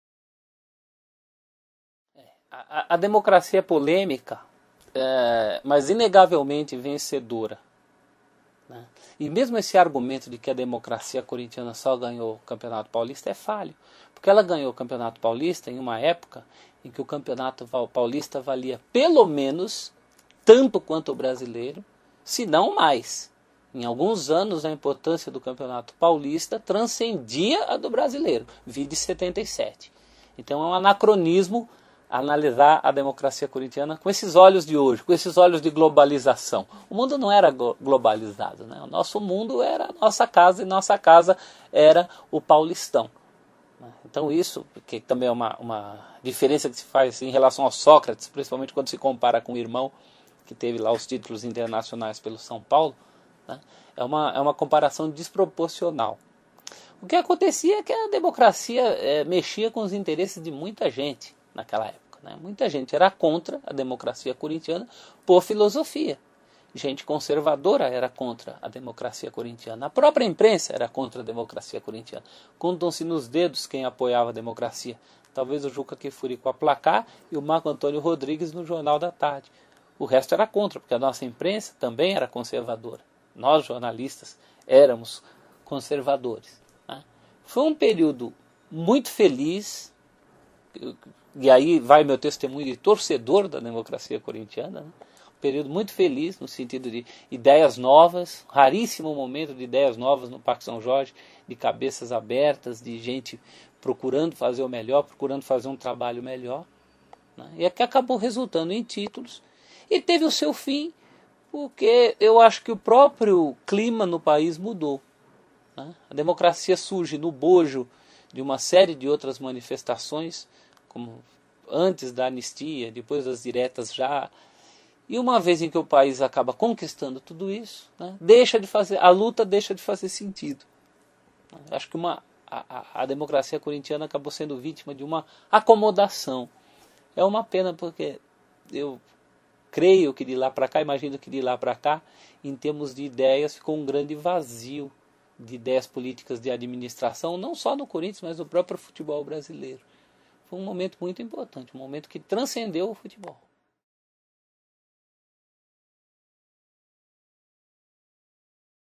DEMOCRACIA VITORIOSA - POR CELSO UNZELTE
O pesquisador Celso Unzelte fala sobre as conquistas da Democracia Corintiana.

CORINTHIANS 100 ANOS, ETERNA PAIXÃO
A Rádio CBN e e Rádio Globo homenageiam o clube pelo seu primeiro centenário. e